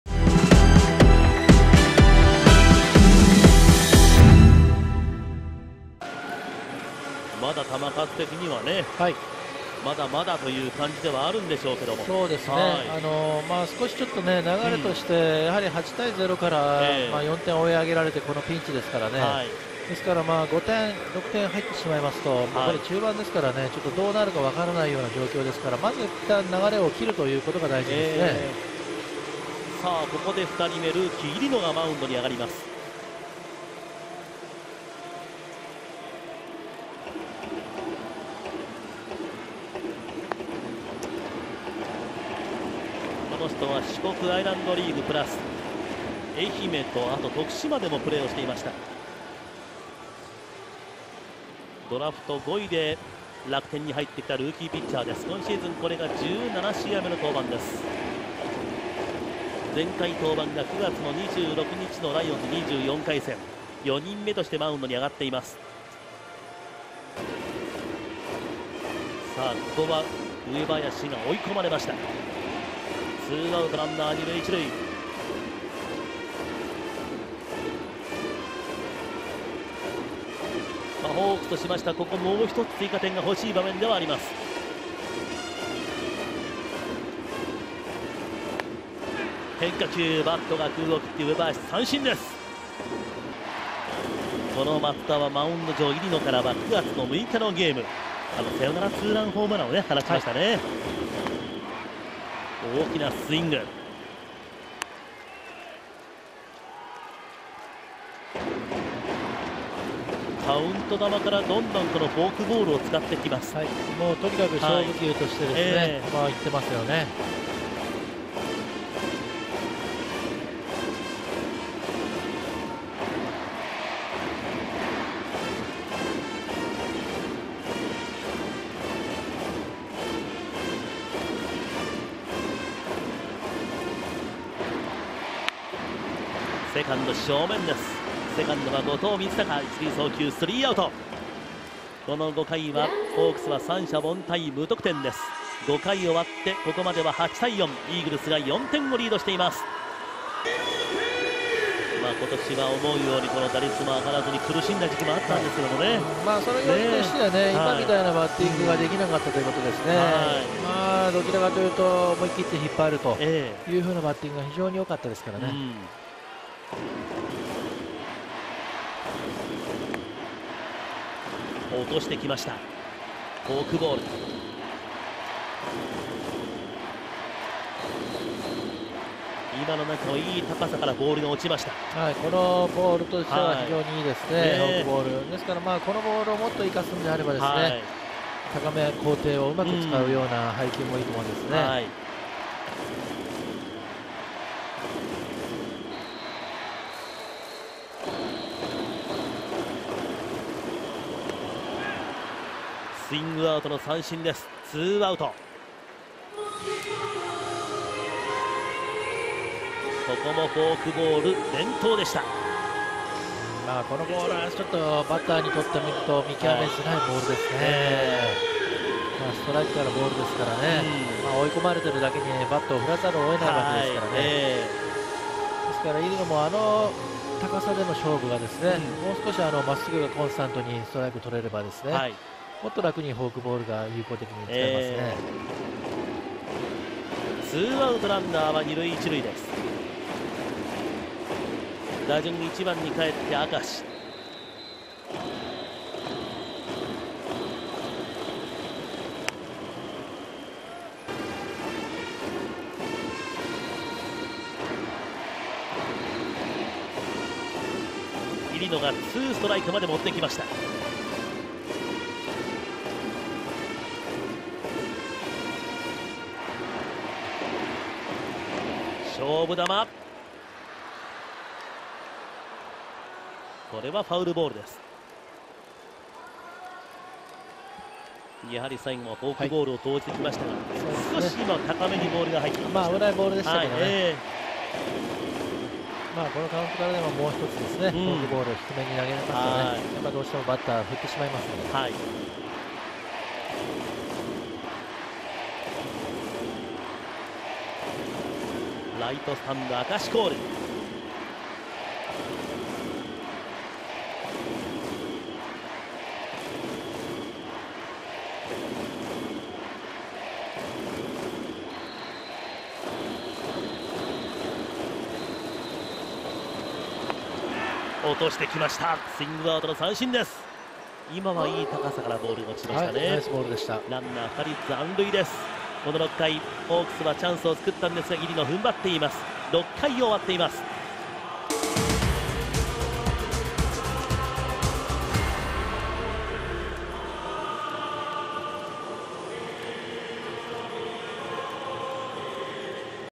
【プロ野球パ】自慢のフォークが冴え渡る！ルーキー入野、好投でプロ初勝利の権利ゲット 2015/09/29 H-E
4回途中で降板のレイに代わってマウンドへ上がったルーキーの入野。フォークボールを軸にした投球で相手の得点を許さず、6回までを投げプロ初勝利の権利を持って降板した。 （9月29日 ソフトバンク-楽天）